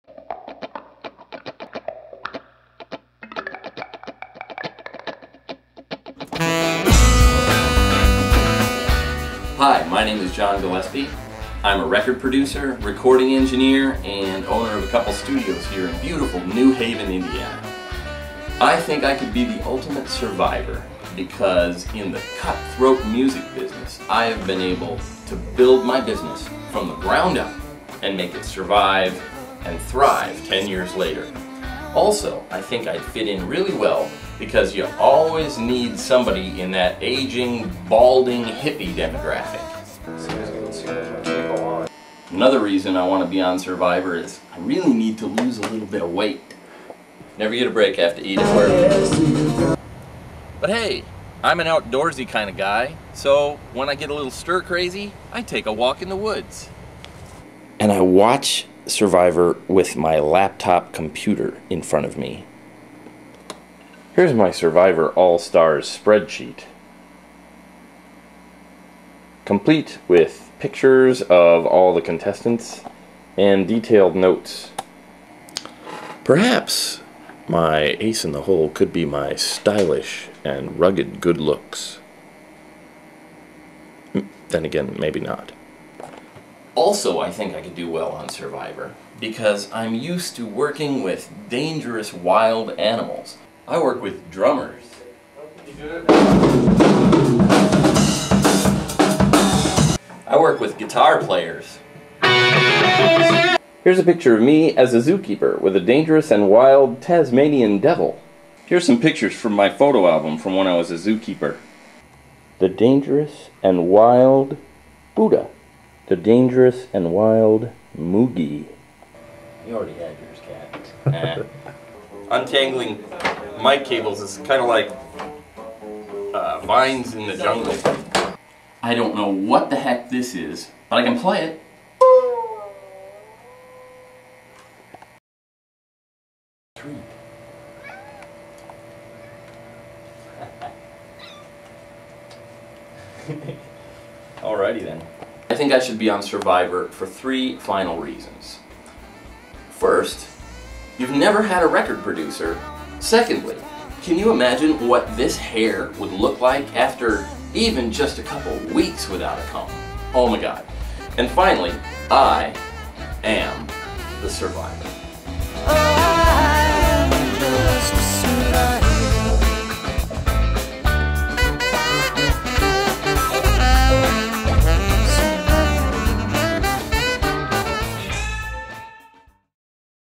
Why Jon Gillespie should be a contestant on Survivor
Audition tape for Survivor, made by Jon Gillespie